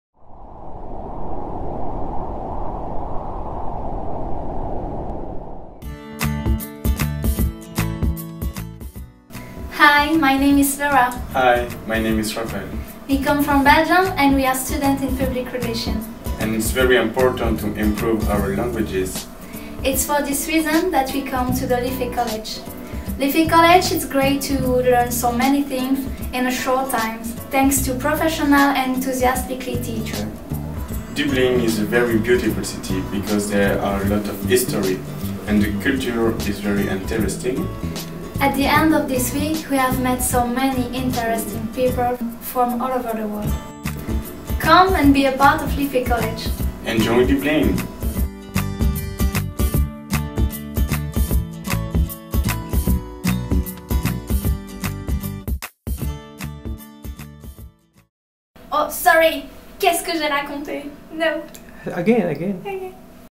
Liffey College Experience - Laura and Raphael & General English Course
Laura Gilot and Raphael Roberto (Belgium) came to Ireland to have an international experience.

If your next stage is to improve your life, challenge yourself and the world can be yours!

Join us and see a whole new world!